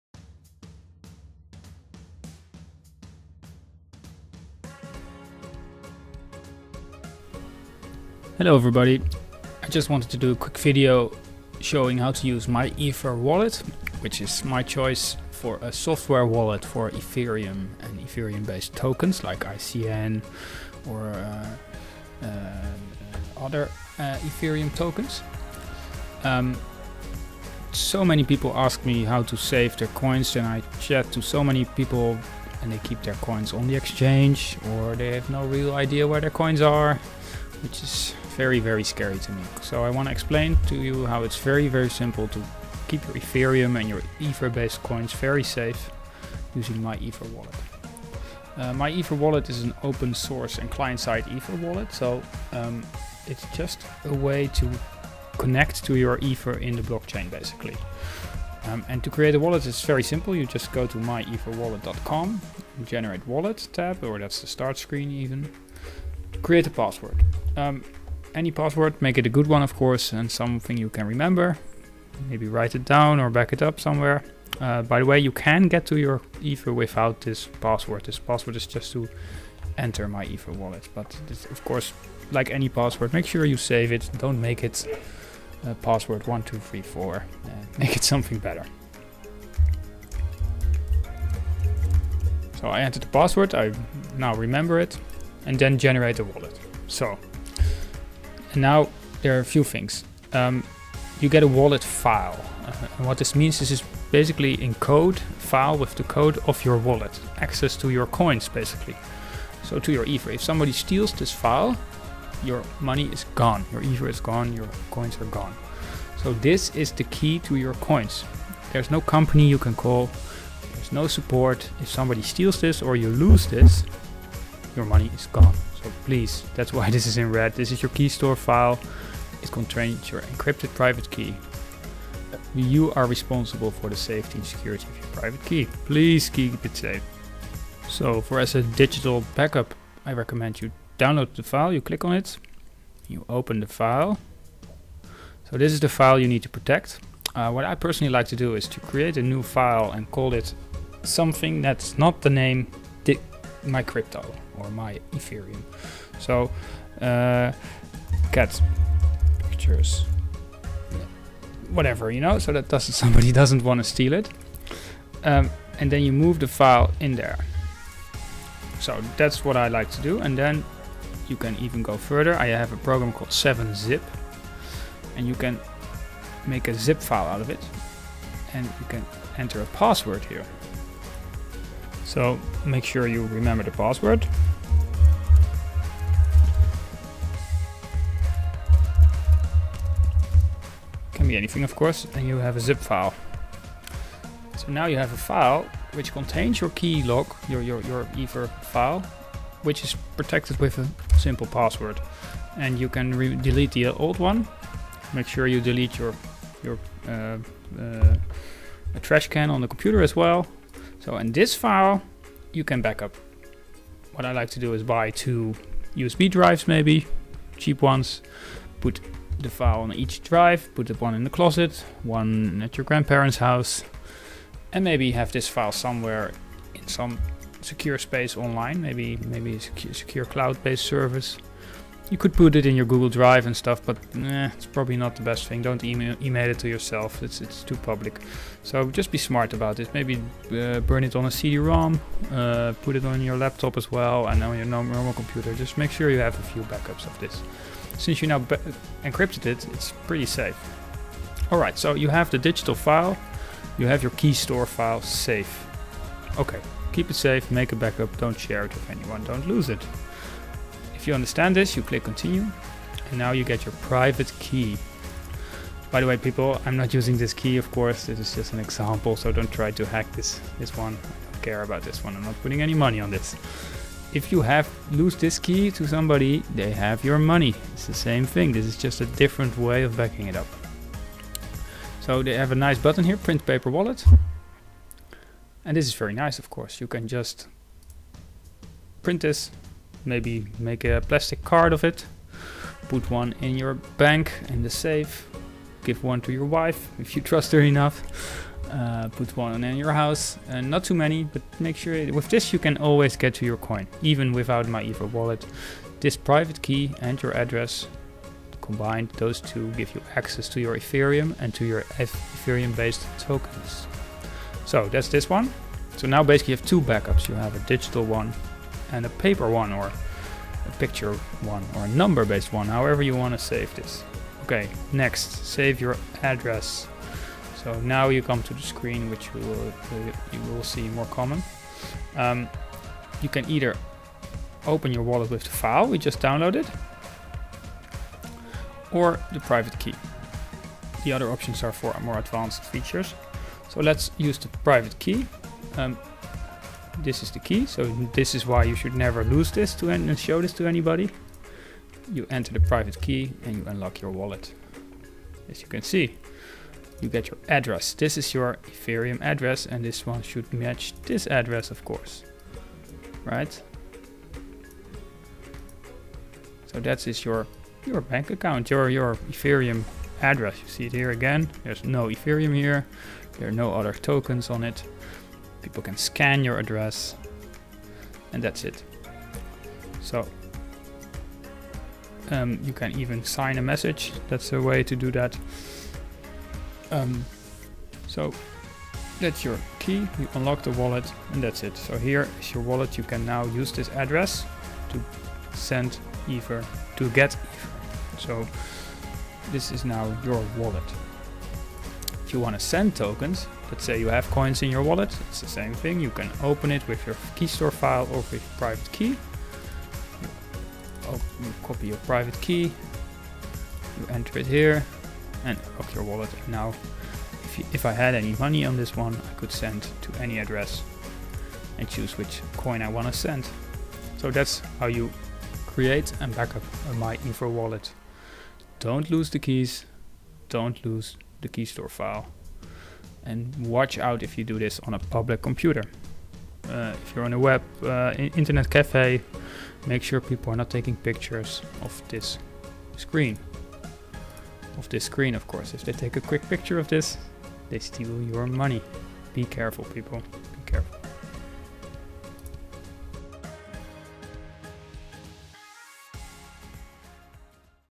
Create and backup a MyEtherWallet
Tutorial on how to safely create a backup a MyEtherWallet for ETH and Ethereum based crypto.